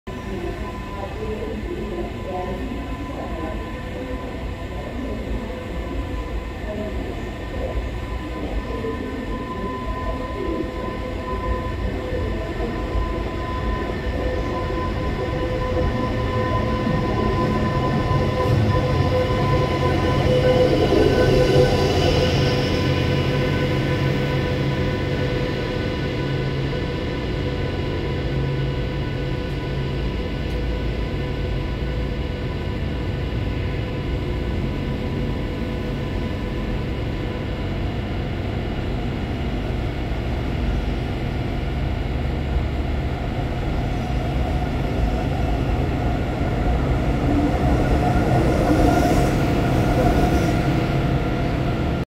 🇩🇪 BR 101 088 mit zug IC 1272 Dresden - Rostock
🇺🇦Електровоз BR 101 088 з поїздом IC 1272 Дрезден - Росток відправляється від станції Берлін
Станція Берлін-Головний ; Німецька залізниця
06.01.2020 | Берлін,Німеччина

🇷🇺Електровоз BR 101 088 з поездом IC 1272 Дрезден - Росток отправляется от станции Берлин
Станция Берлин-Главный; Немецкая железная дорога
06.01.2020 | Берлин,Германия

🇩🇪 BR 101 088 mit zug IC 1272 Dresden - Rostock fährt Berlin HBF ab
Berlin Hauptbahnhof; DeutschBahn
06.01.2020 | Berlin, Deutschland
Der Autor des Videos - wl/trains

🇬🇧 BR 101 088 with train IC 1272 Dresden - Rostock departing from Berlin Central station
Berlin Central Station; DeutschBahn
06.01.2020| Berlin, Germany
The author of the video - wl/trains